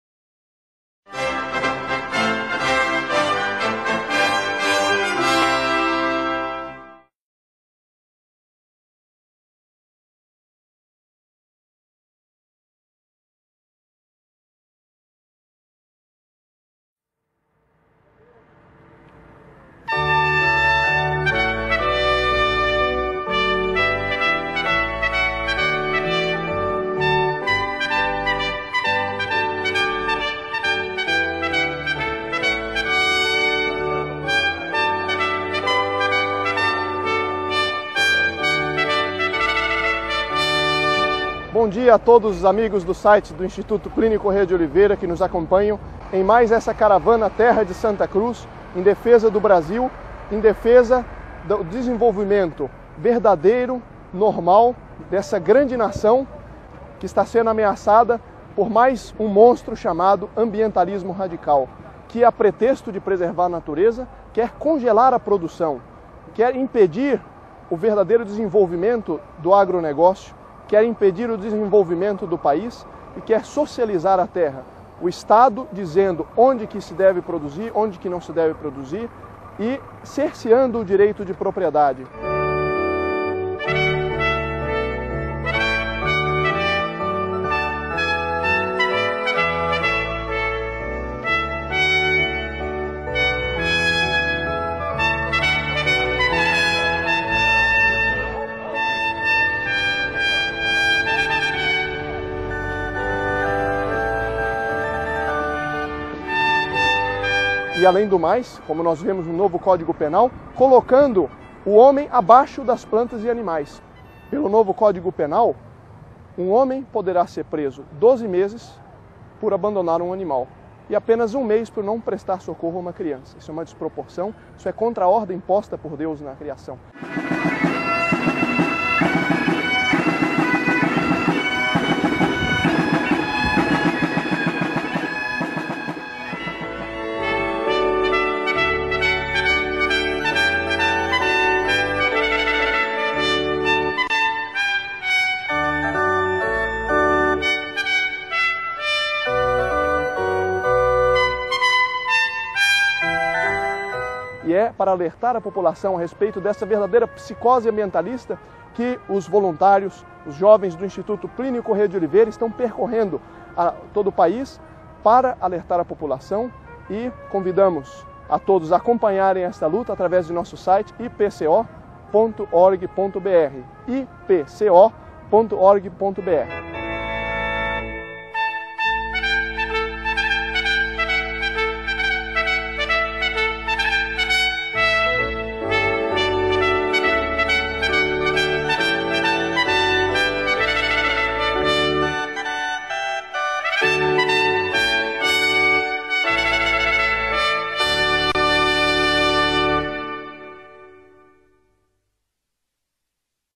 36 jovens versus Psicose Ambientalista
Mais uma vez os caravanistas do Instituto Plinio Corrêa de Oliveira percorrem o Brasil em defesa da civilização cristã, esclarecendo a população sobre os perigos do ambientalismo radical.